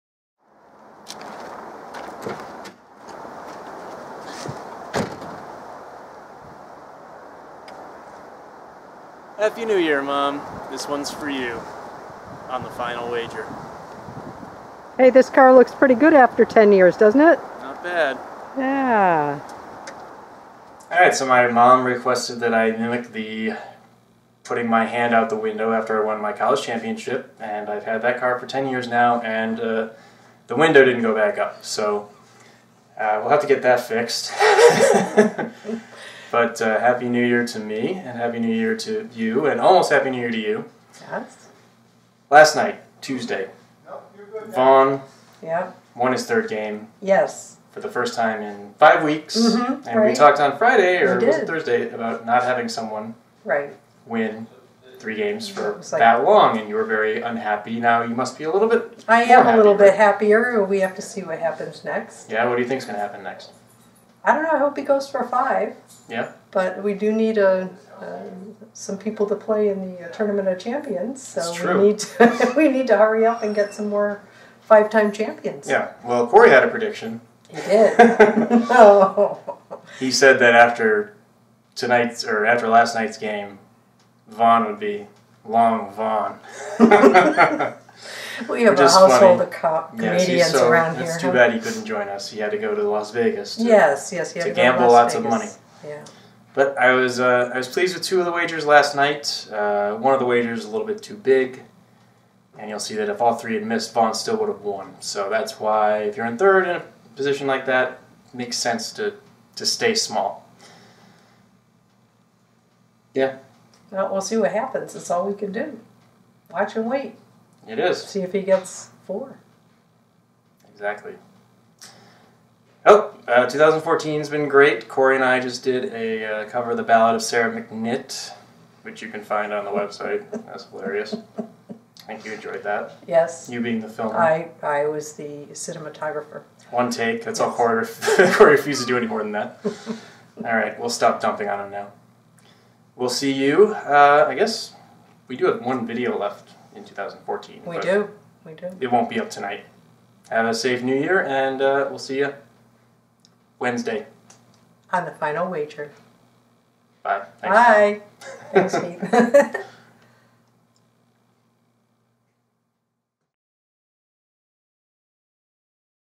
The Final Wager – Tuesday, December 30, 2014
2003 Jeopardy! College Champion Keith Williams (and his mother, Katherine) breaks down the wagering for the match Vaughn Winchell vs. Marcia Kuipers vs. Dan Eling, which first aired December 30, 2014.